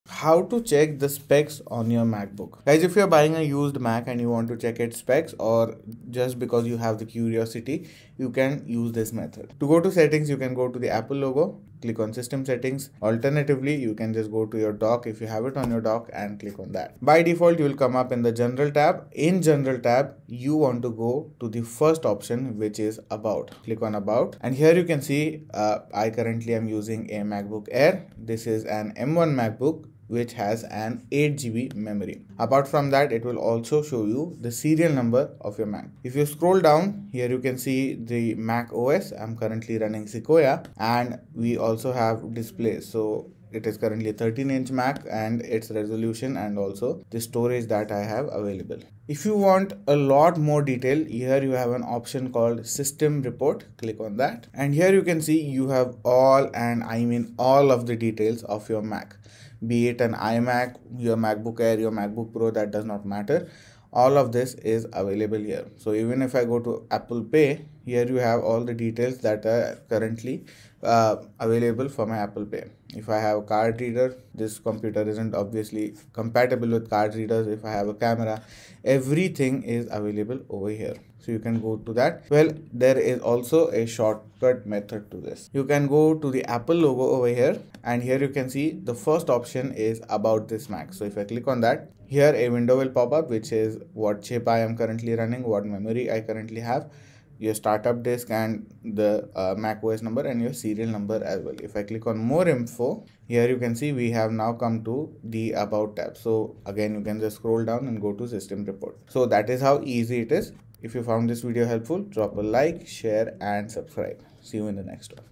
How to Find Specs on Mac? How to Check details in Macbook Pro, Air, Mini & iMac
How to find details in a Macbook? How to see the serial number in a mac? In this video, I have explained and demonstrated the step-by-step guide on how you can access the details about your device in macOS. Watch the video, and let me know your queries in the comment section.